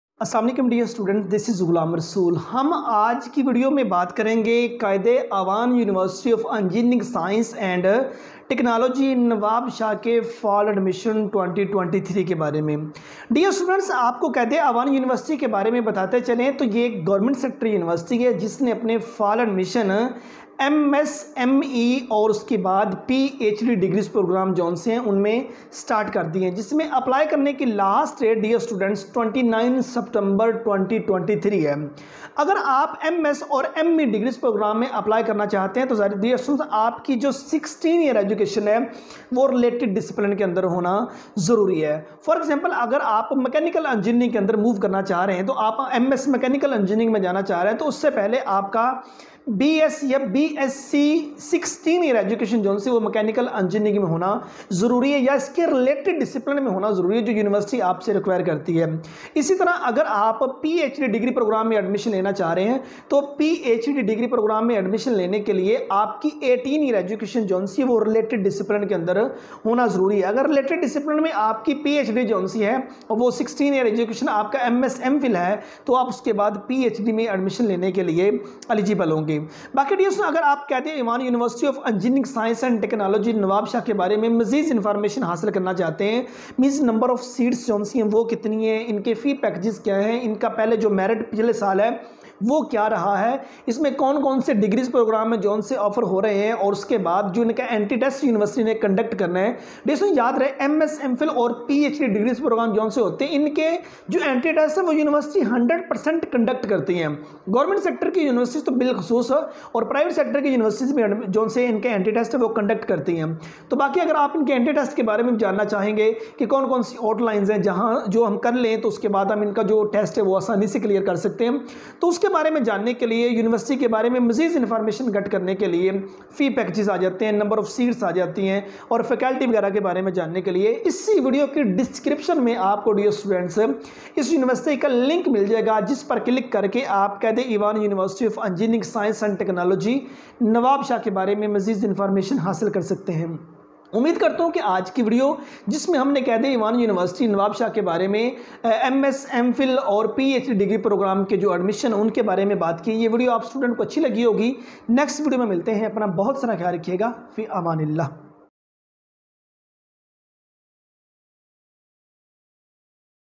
MS, ME & Ph.D Admission 2023 | Quaid-e-Awam University | قائدایوان یونیورسٹی
Quaid-e-Awam University of Engineering, Sciences & Technology (QUEST), Nawabshah
MS, ME & PhD Degree Programs 2023
Sector: Govt
Last Date to Apply:29, September, 2023